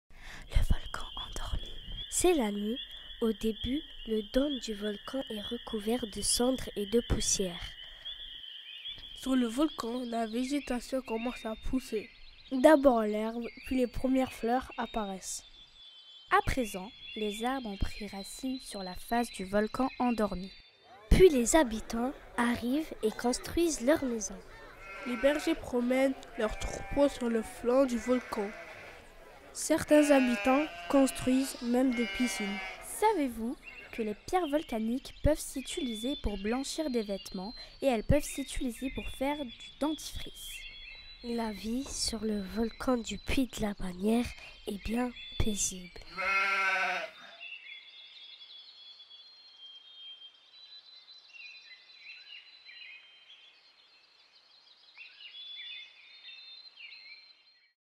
Atelier : le volcan endormi
Vidéo co-réalisée avec Marie-Noëlle Battaglia pour une classe de CM2 de l'école Peyssonnel 1 à Marseille dans le cadre du dispositif "Journalistes en résidence", financée par la Fondation de France et la Drac Paca.